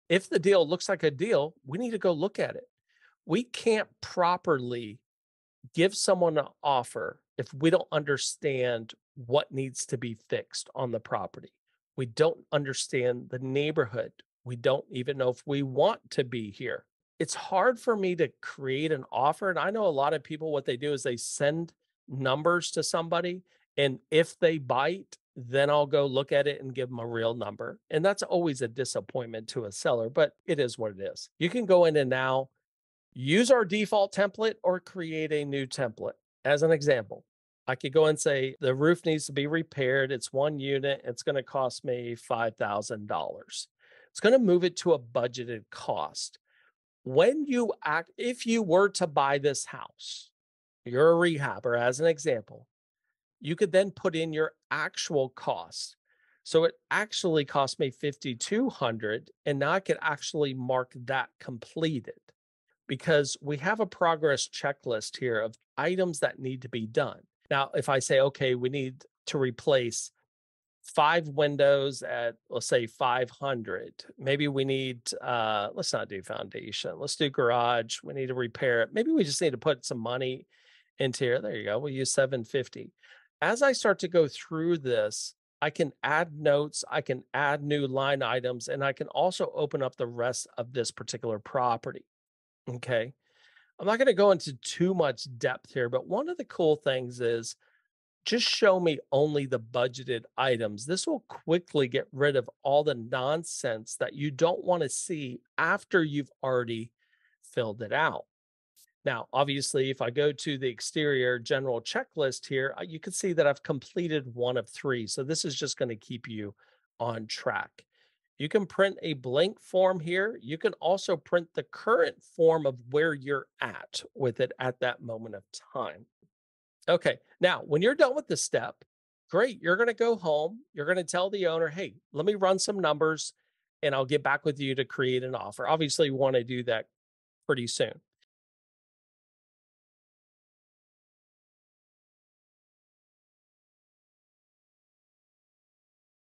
Property Inspection Checklist for Real Estate Investors | REIPro® Software
Learn the essential step that will help you determine the total cost for repairs on any property inspection checklist - REIPro's Executable Step #3! This video guide explains how to quickly and accurately enter numbers into a property inspection form to get a realistic estimate of repair costs. Get the information you need today with this step-by-step tutorial from REIPro - the number one real estate investment software!